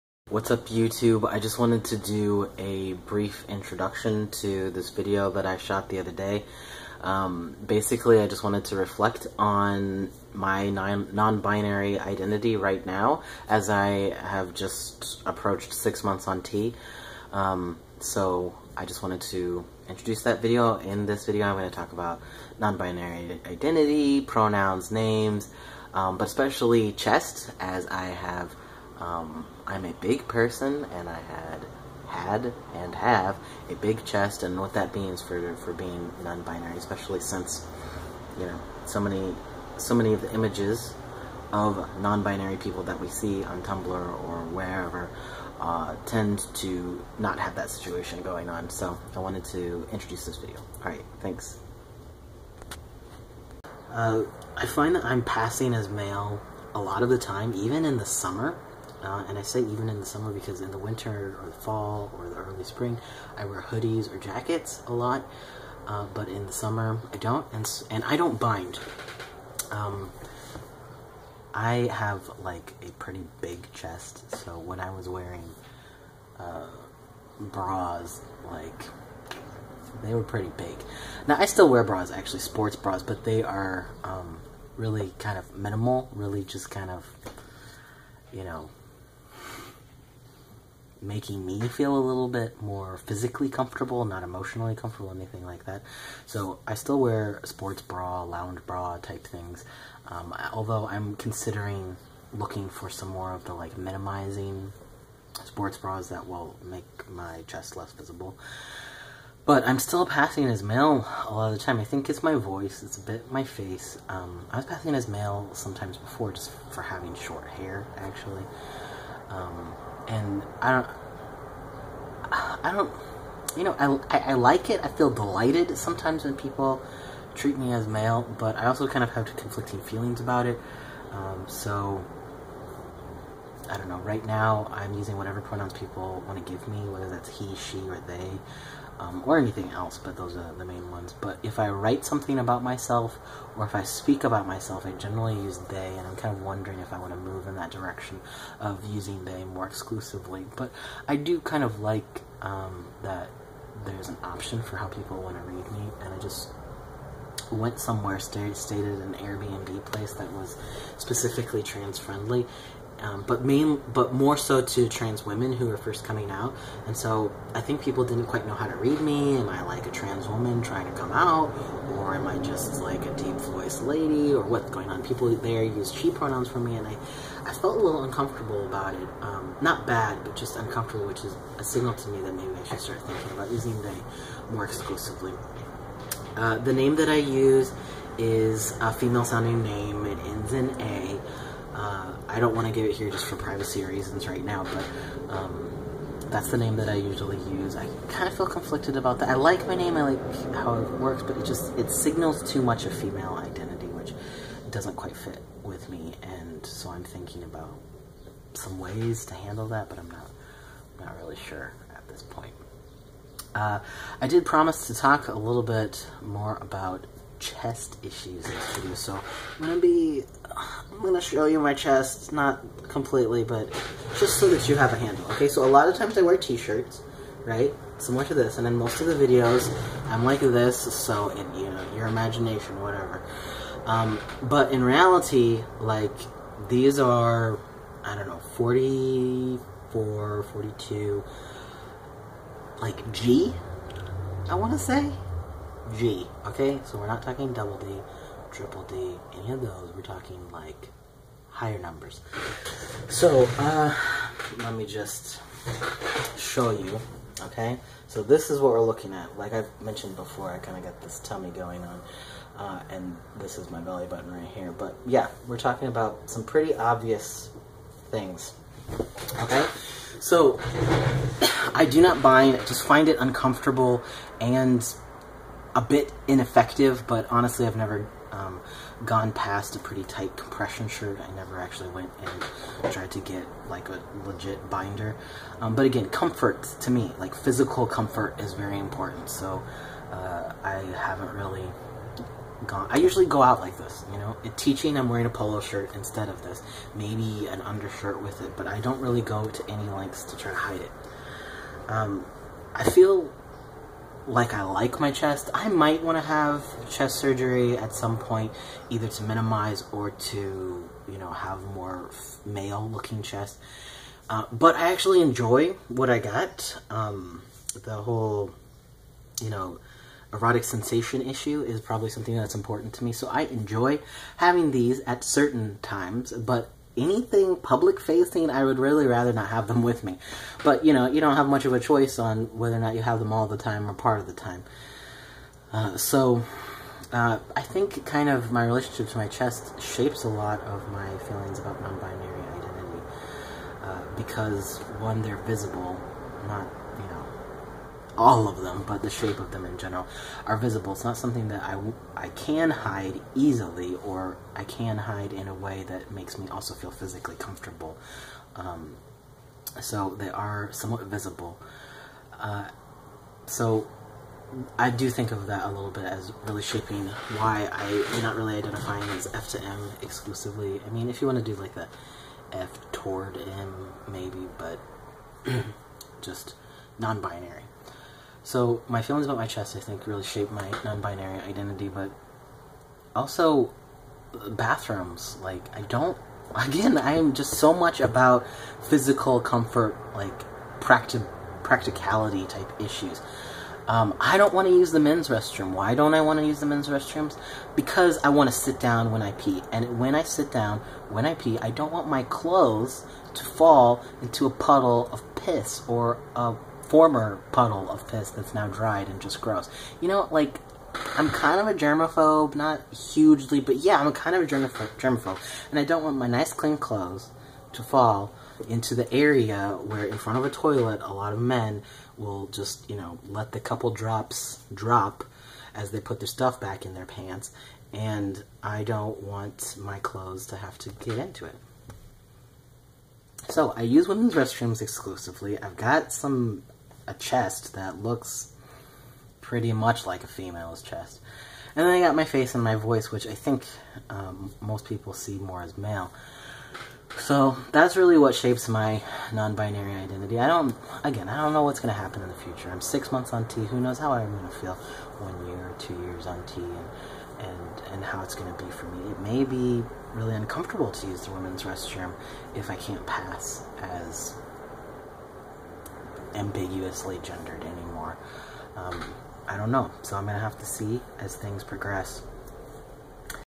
Nonbinary identity and my (big) chest [6 months on T]
I talk a little about why "nonbinary trans" fits for me right now vs. "FTM" or just "trans/transgender." Specifically, I talk about my name, pronouns, bathrooms, and my (rather large) chest, which includes discussion of being fat, choosing not to bind, bras, visibility, erotic sensation, and physical comfort.

About me: I'm a nonbinary trans FTM-ish POC documenting my experiences and the latest transition in my life: HRT.